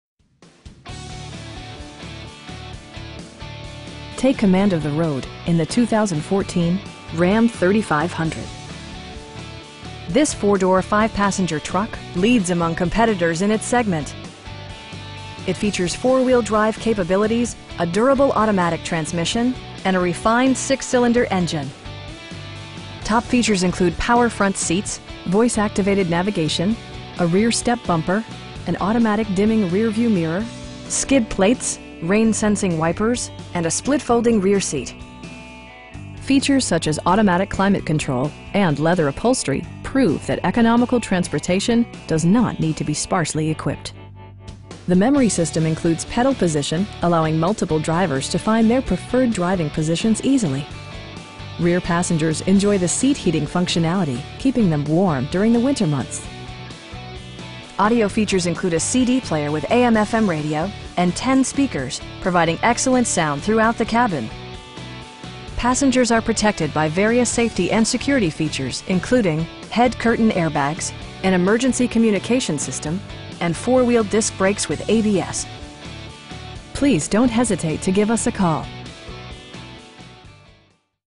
2014 Ram 3500 Laramie in Covington, GA 30014
Ginn Chrysler Jeep Dodge
5190 U.S. 278
in Covington, GA 30014

Take command of the road in the 2014 Ram 3500. This 4 door, 5 passenger truck leads among competitors in its segment. It features four-wheel drive capabilities, a durable automatic transmission, and a refined 6 cylinder engine. Turbocharger technology provides forced air induction, enhancing performance while preserving fuel economy. Top features include power front seats, voice activated navigation, a rear step bumper, an automatic dimming rear-view mirror, skid plates, rain sensing wipers, an overhead console, and a split folding rear seat. Features such as automatic climate control and leather upholstery prove that economical transportation does not need to be sparsely equipped. The memory system includes pedal position, allowing multiple drivers to find their preferred driving positions easily. Rear passengers enjoy the seat heating functionality, keeping them warm during the winter months. Audio features include a CD player with AM/FM radio, steering wheel mounted audio controls, and 10 speakers, providing excellent sound throughout the cabin. Passengers are protected by various safety and security features, including: head curtain airbags, front SIDE impact airbags, traction control, brake assist, a panic alarm, an emergency communication system, and 4 wheel disc brakes with ABS. Various mechanical systems are monitored by electronic stability control, keeping you on your intended path. We have the vehicle you've been searching for at a price you can afford. Please don't hesitate to give us a call.